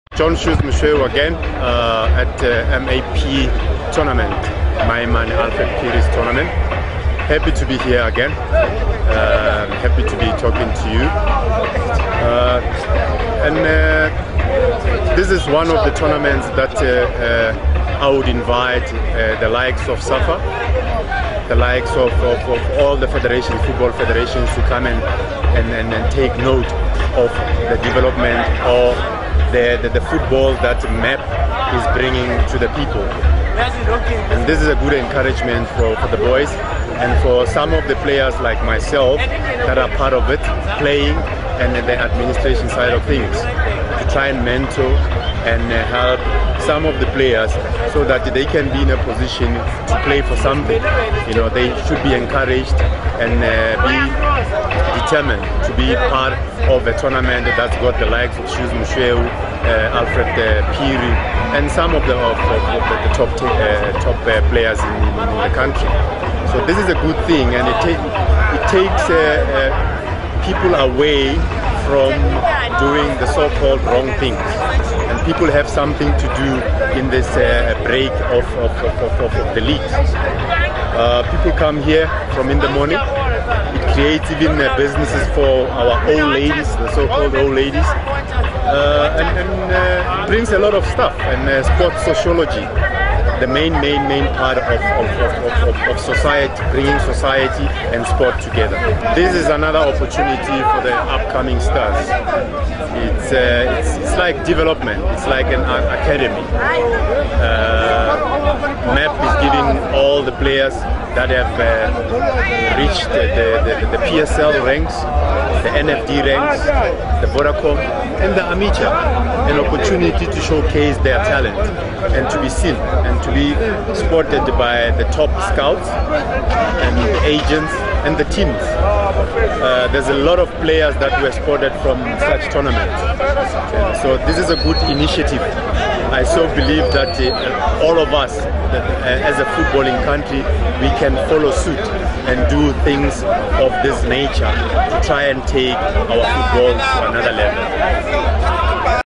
John "Shoes" Moshoeu - Soccer Legend
Comments from one of South Africa's legends of the game.